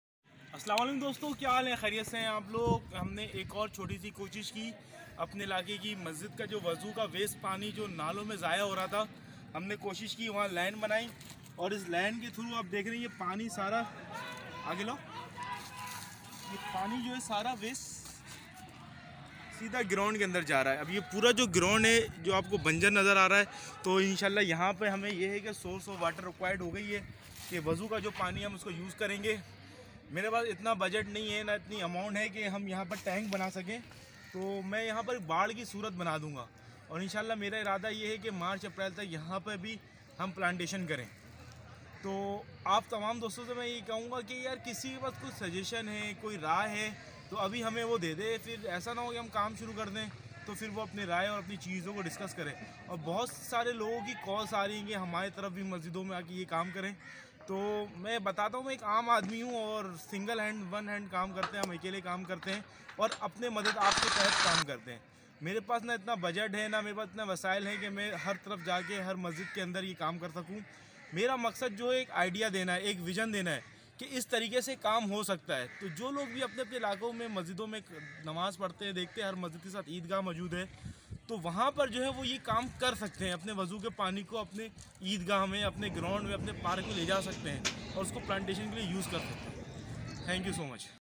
Using Ablution Water In Masjid for Plantation Save Water Save Environment
The Tree Man Of Pakistan..

Shahroz Siraj..

Founder / PresidentGreen Hands Welfare Organization..

Plantation Awareness Campaign For Youth..